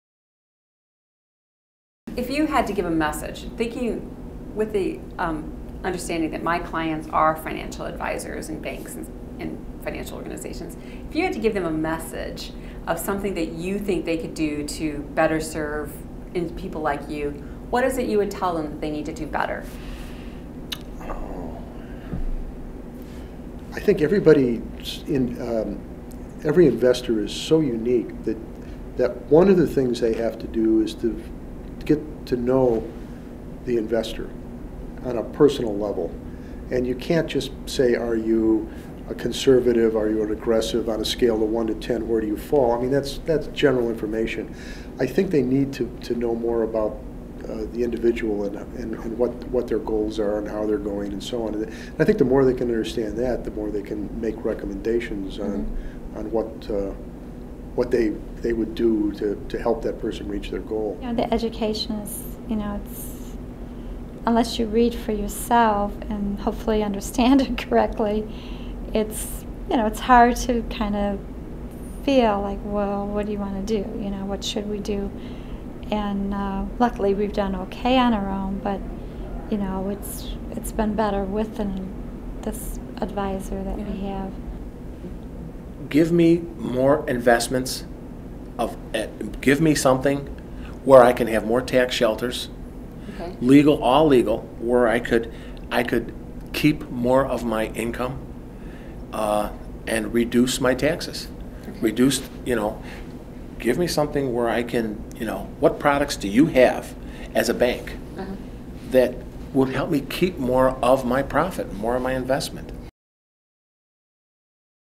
What can financial services providers do you for you as an investor?
Spectrem Focus Group Research.  What can financial services providers do to help you as an investor?